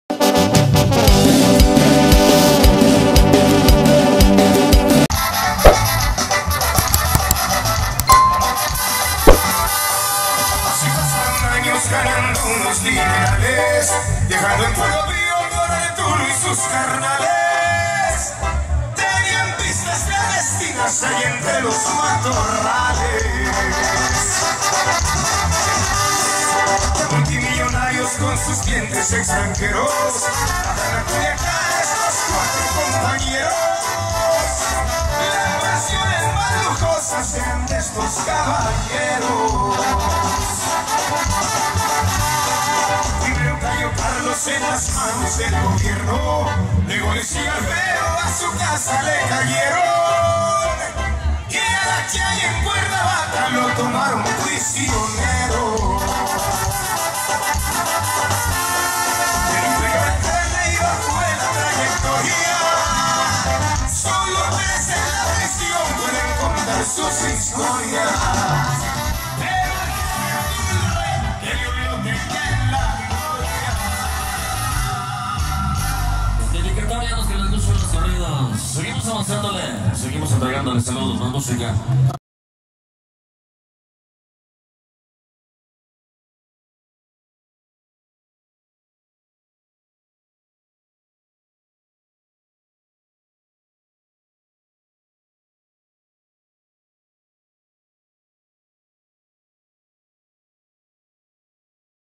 Esta mujer de color vino lucio BELLA y ESPECTACULAR en la pista de baile
Amigos aqui les dejamos este bonito bailazo en la Expo Feria del Gallo 2020 para que lo disfrutes en tu casa...